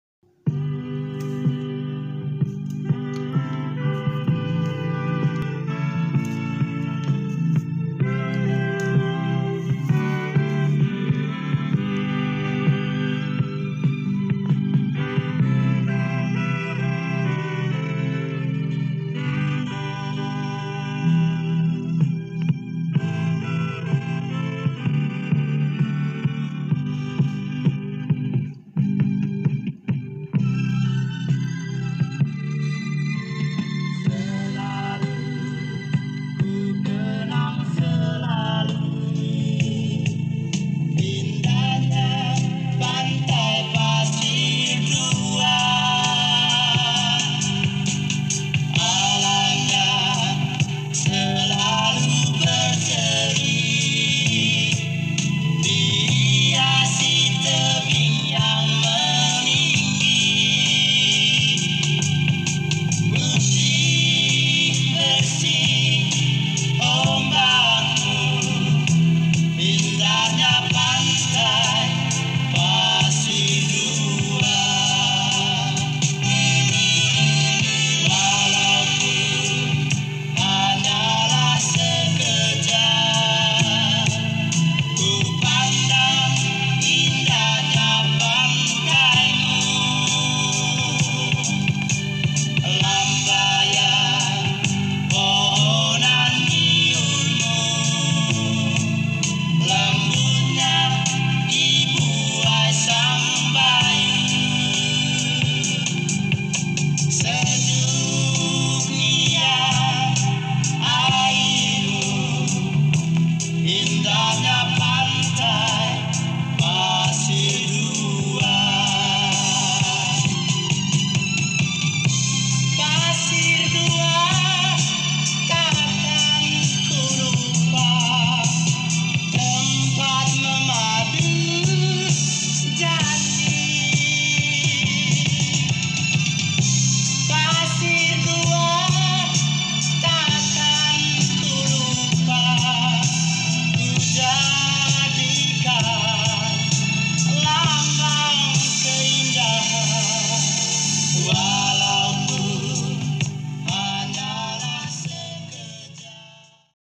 Sophie di Pantai Soryar Biak Timur
Practice my little daughter for swimming.